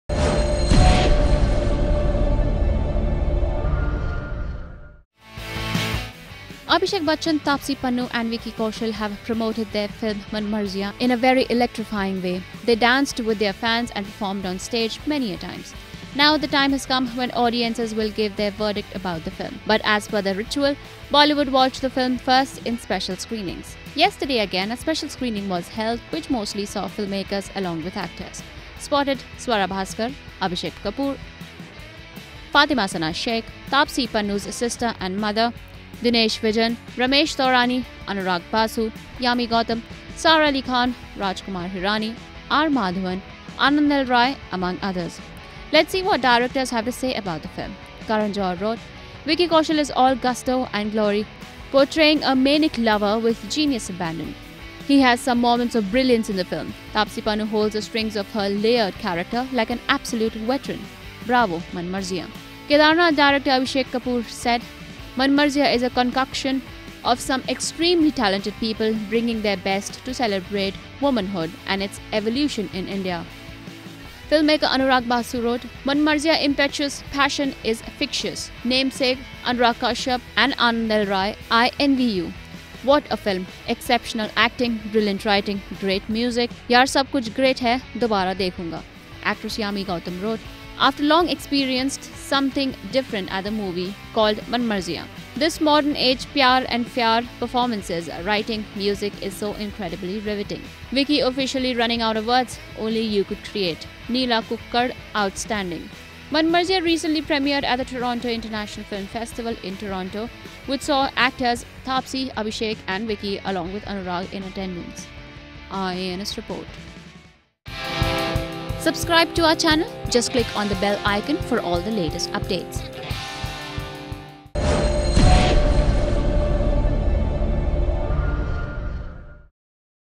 Filmmakers VERDICT on ‘Manmarziyaan’ | Abhishek, Taapsee & Vicky
Abhishek Bachchan, Taapsee Pannu and Vicky Kaushal have promoted their film “Manmarziyaan” in a very electrifying way. They danced with their fans and performed on stage many a times. Spotted Swara Bhasker, Abhishek Kapoor, Fatima sana sheikh, Taapsee Pannu’s sister and mother, Dinesh Vijian , Ramesh Taurani, Anurag Basu, Yami Gautam, Sara Ali Khan, Rajkummar Hirani, R Madhvan, Anand L rai.  "Manmarziyaan" recently premiered at the Toronto International Film Festival (TIFF) in Toronto, which saw actors Taapsee, Abhishek and Vicky along with Anurag in attendance.